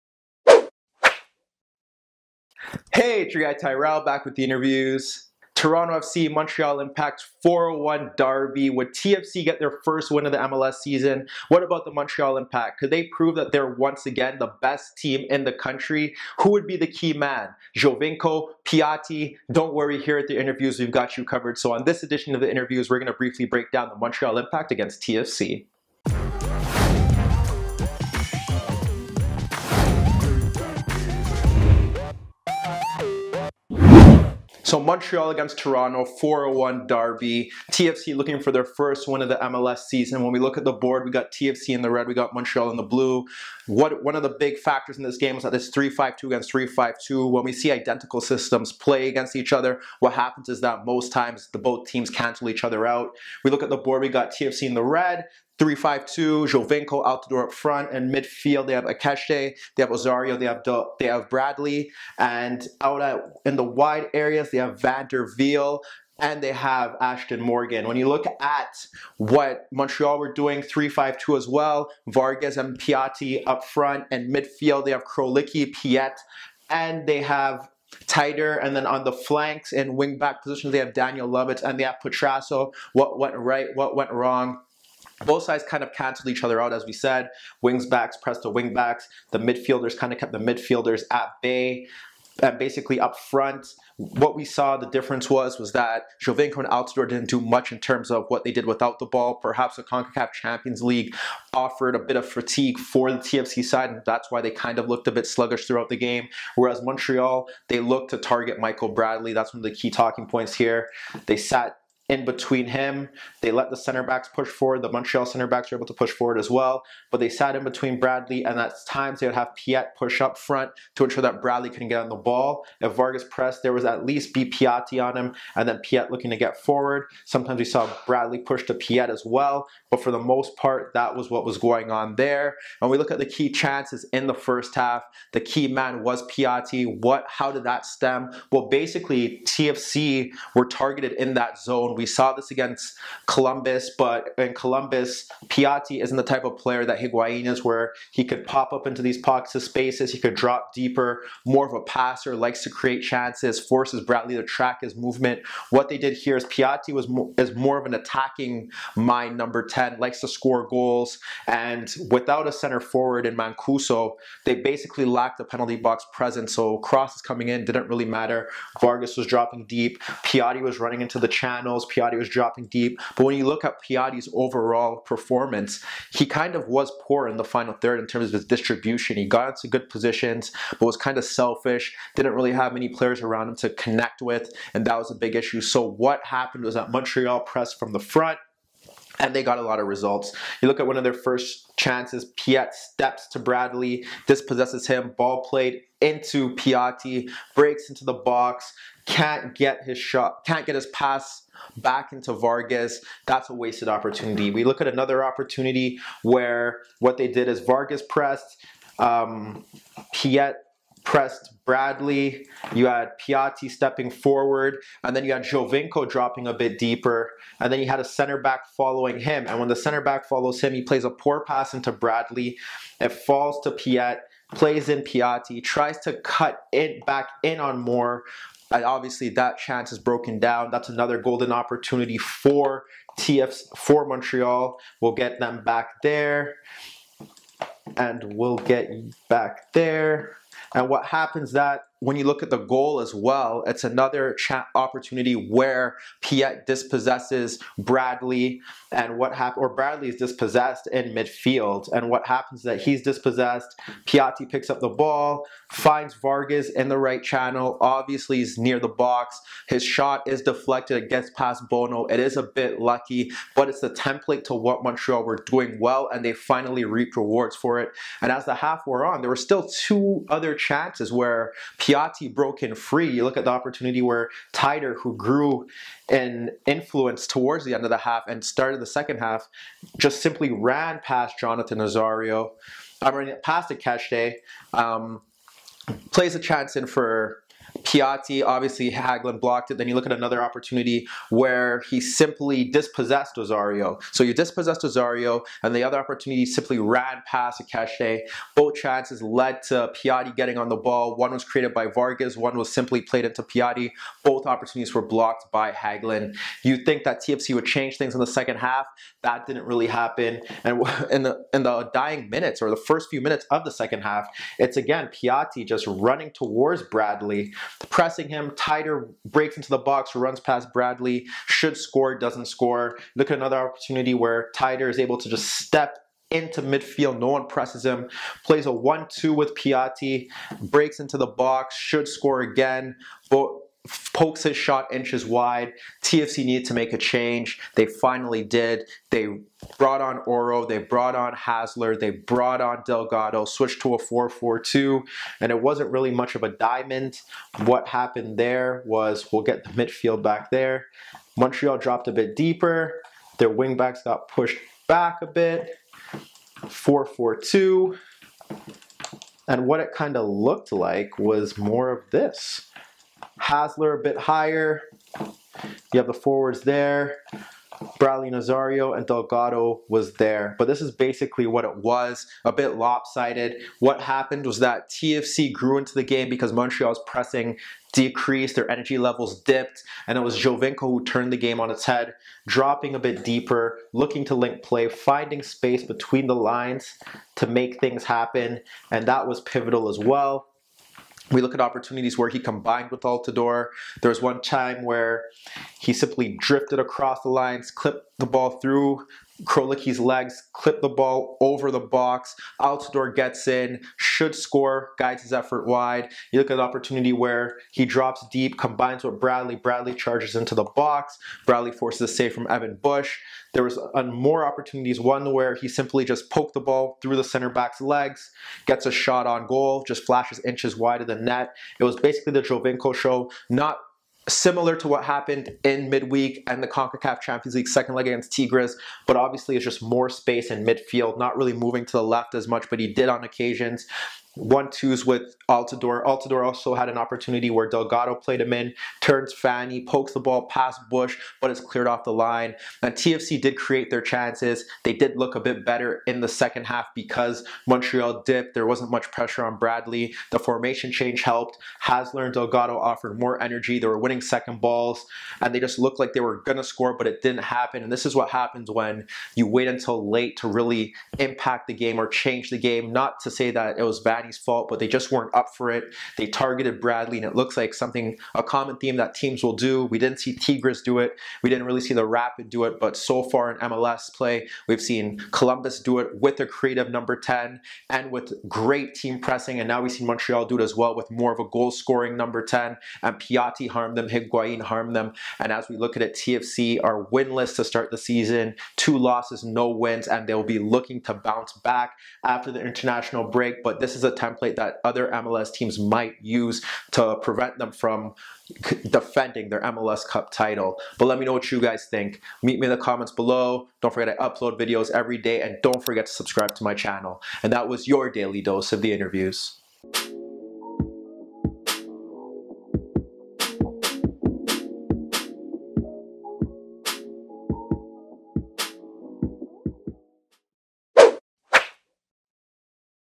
Montreal Impact 1-0 Toronto FC l Tactical Analysis l The InnerViews l
On this episode of The InnerViews, Tyrrell breaks down Toronto FC's disappointing lose to rivals Montreal. Ignacio Piatti's first half goal gave Montreal Impact the eventual victory over Toronto FC.

Should Toronto FC be concerned with their slow start to their MLS season?

Who should start at right wing-back for Toronto FC? Auro Jr or Van Der Wiel?

Should Toronto FC continue to play with a three man back-line?

Let me know what you guys think in the comments below!